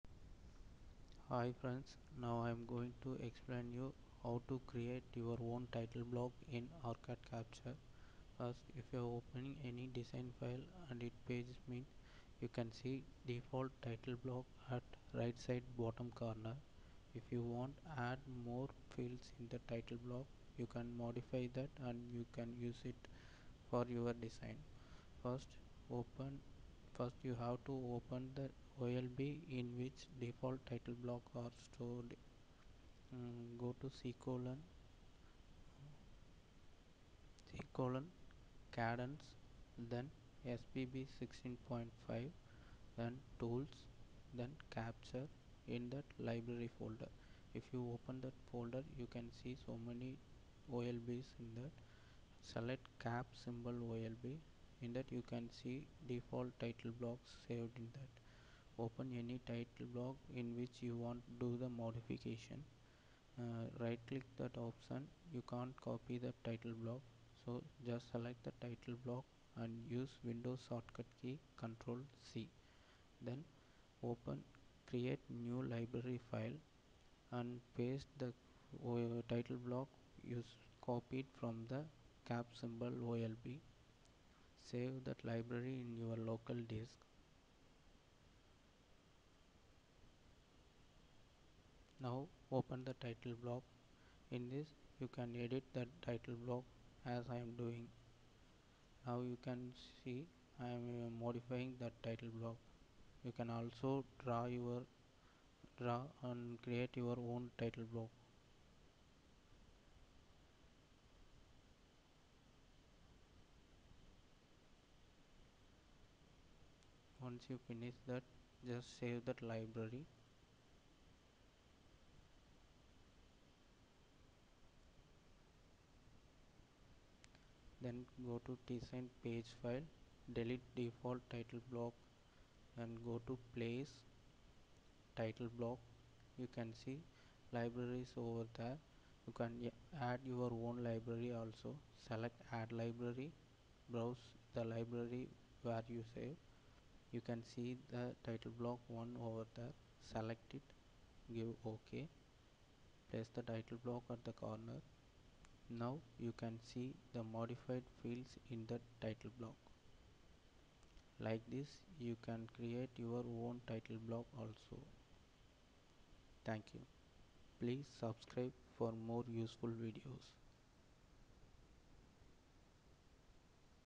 How To Create Title Block in Orcad Cature (Allegro)
How to create your own title block in Orcad Cature (Allegro) also to modify your own title block in Orcad Cature (Allegro)